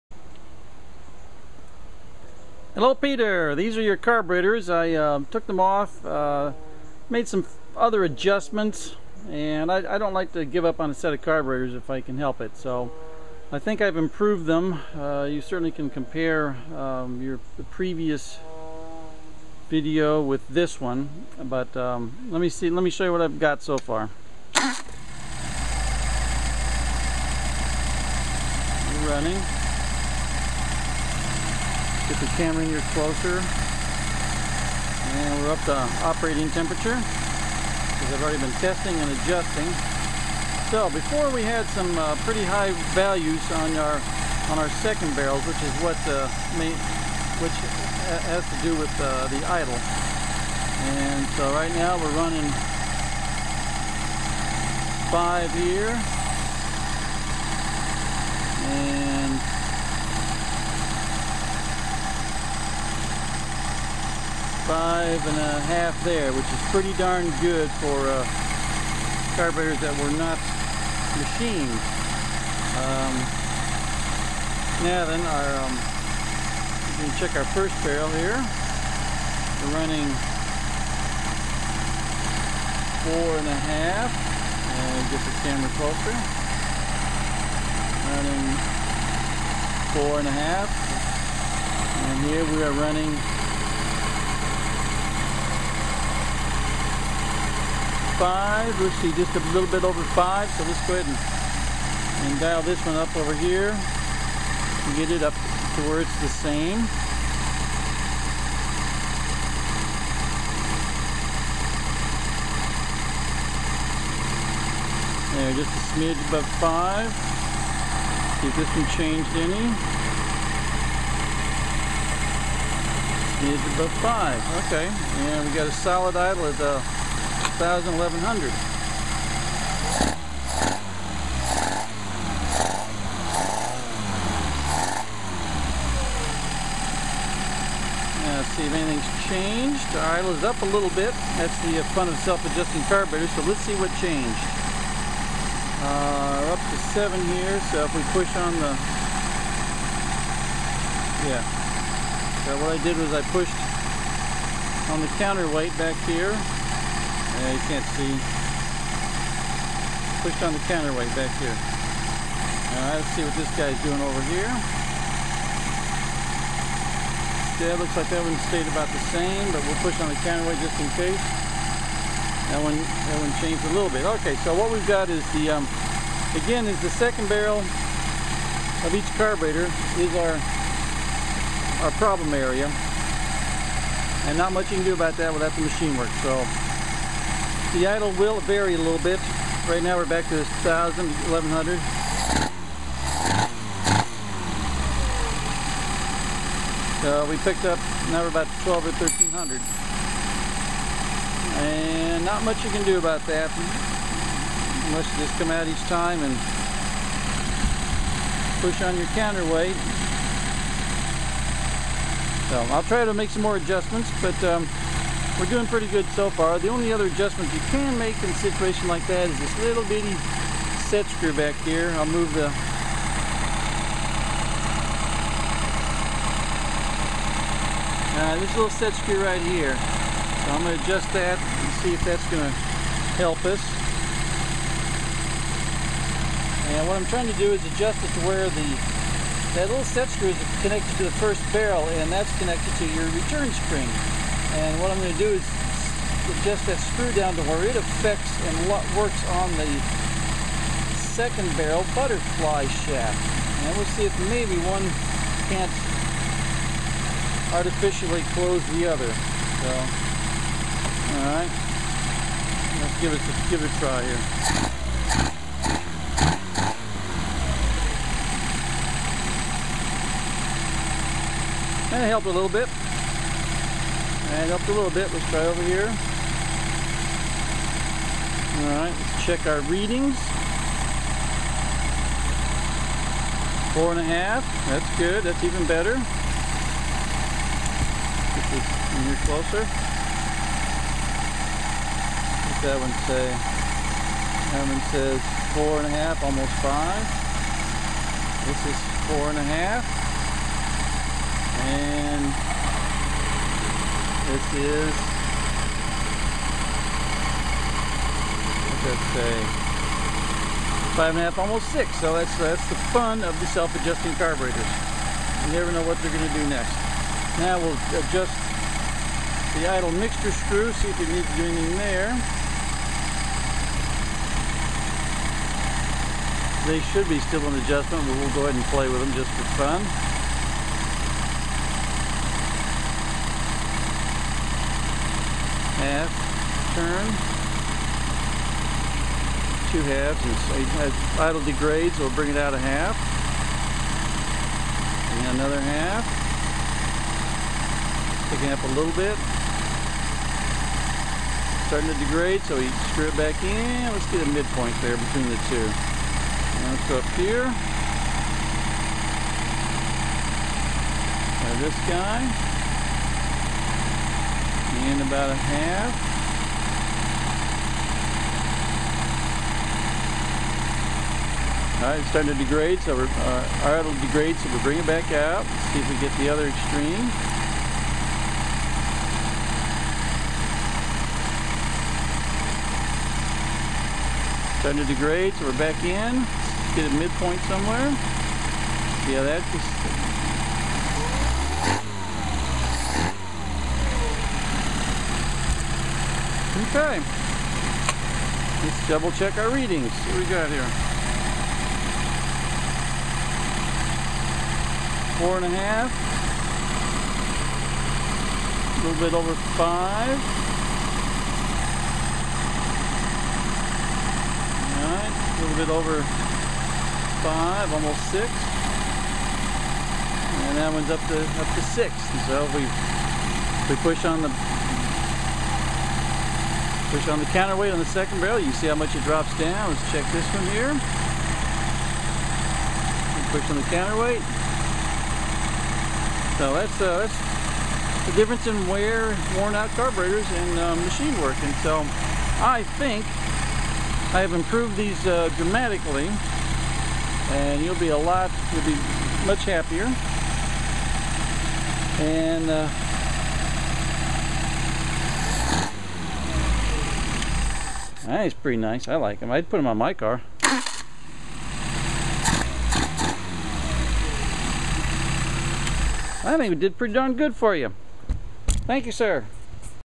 Second test of Peter's 190SL carbs
I just don't like to give up on carbs if I think I can improve their operation.  That is why I took Peter's carbs off after the 1st test and made some more improvements.  I think they are as good as they are going to get.